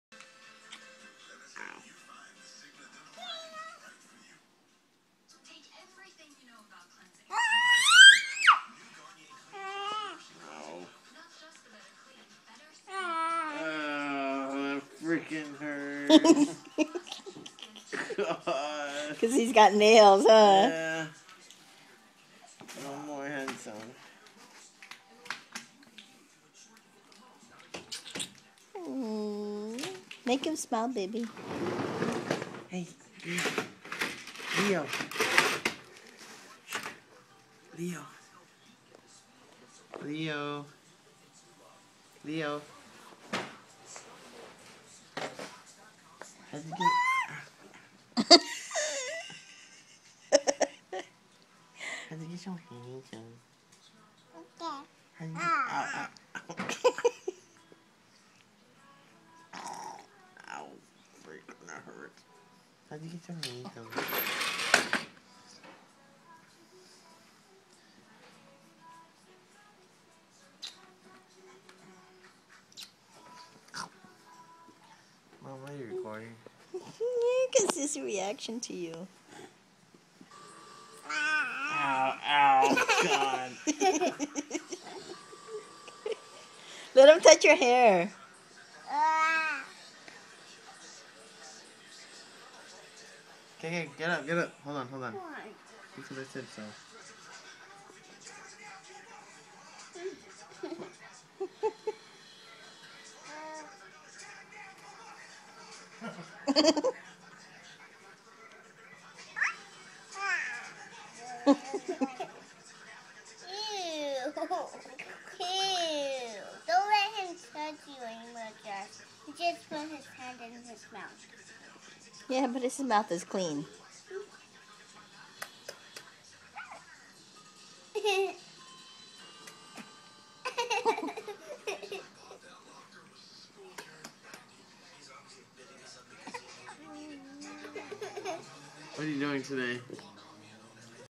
Leo
Leo interacting with his Uncle Joshua